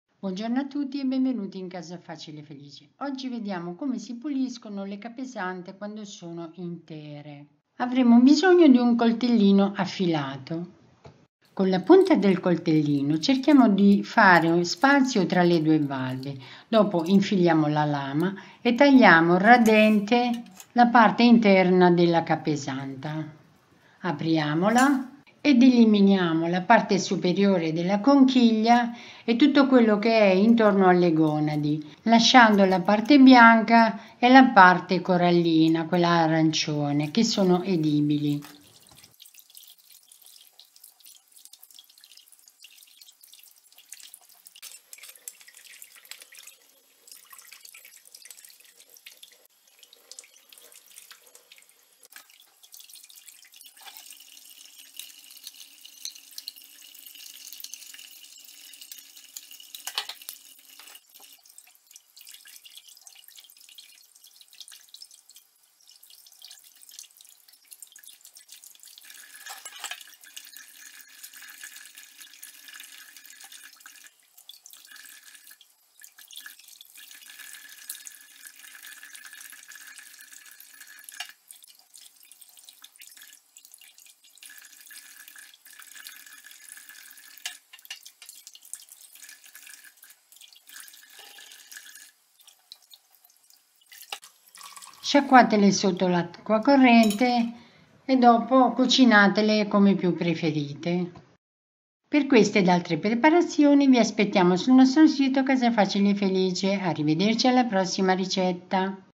Come pulire le capesante intere.
Metodo semplice per pulire le capesante intere in modo da avere a disposizione solo le gonadi, parte bianca e parte arancione edibili.